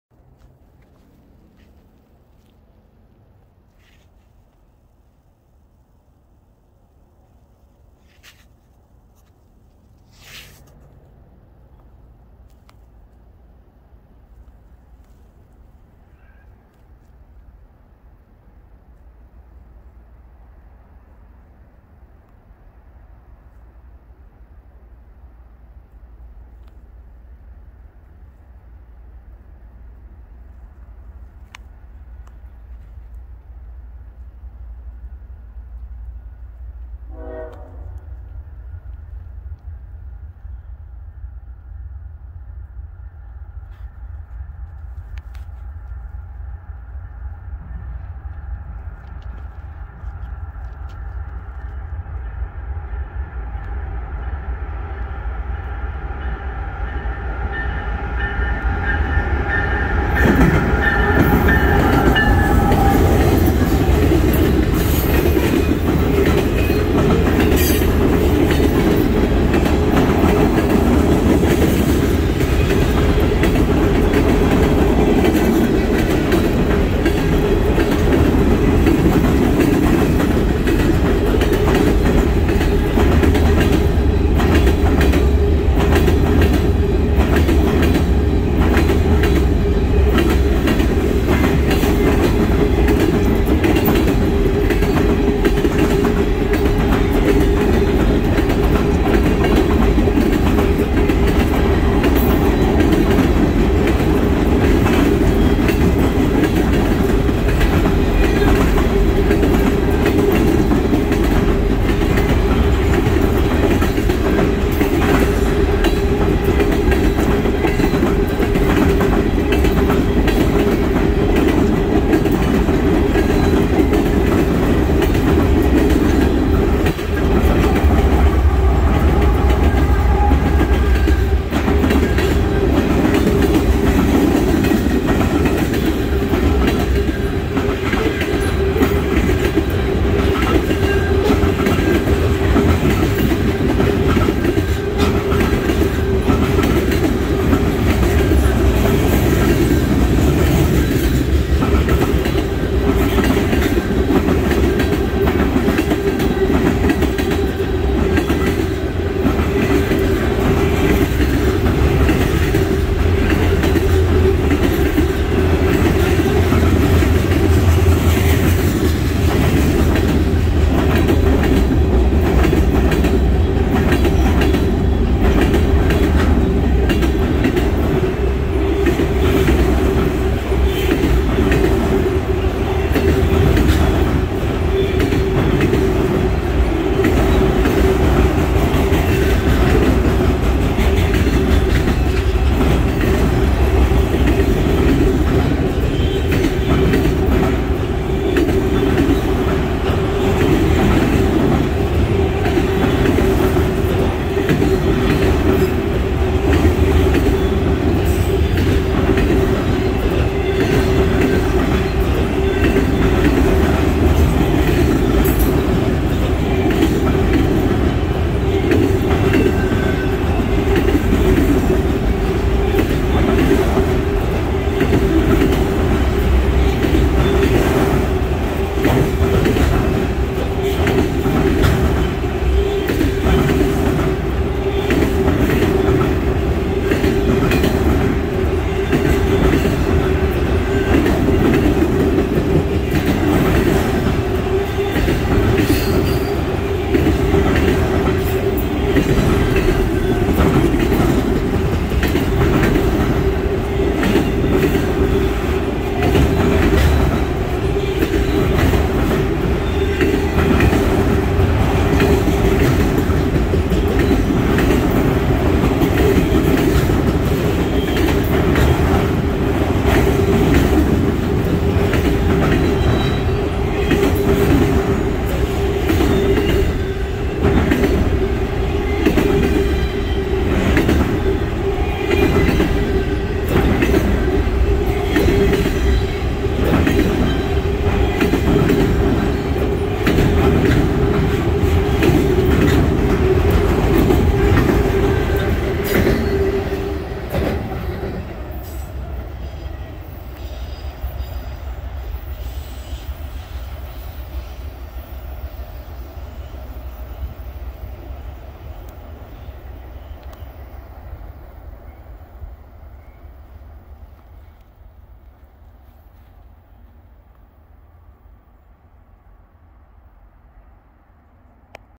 Norfolk Southern 8001 leads a 132 car manifest at Lewistown, PA (12/12/2022)
18N, running heavy on sludge containers and autoracks.